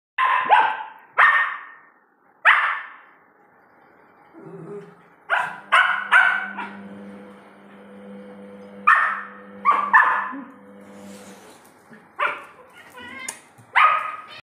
Cat and Dog Fighting😂🥰
വീട്ടിനുള്ളിൽ കയറിയ പൂച്ചയെ തുരത്തുന്ന  ബെല്ല &ലിയോ